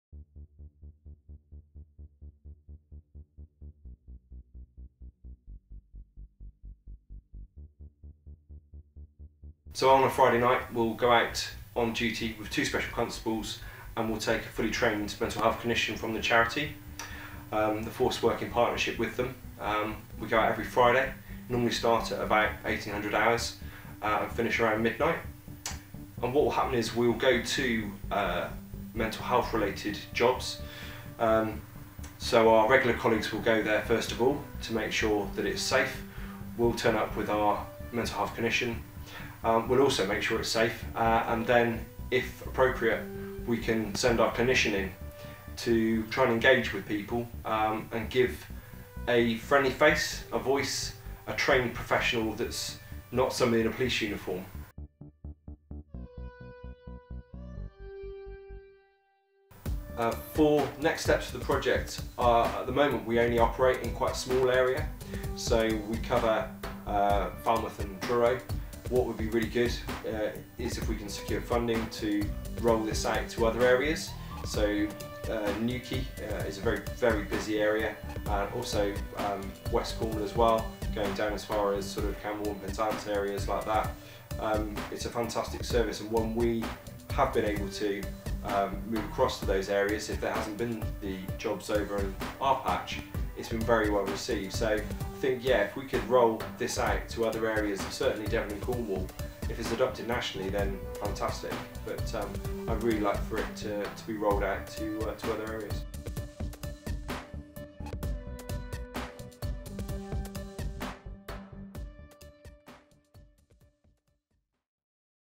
G999 Team | College of Policing Partnership and Collaboration Award Winner
Winners from the 2019 to 2020 College of Policing Awards ‘Recognising professional excellence and a quiet brilliance’ across UK policing.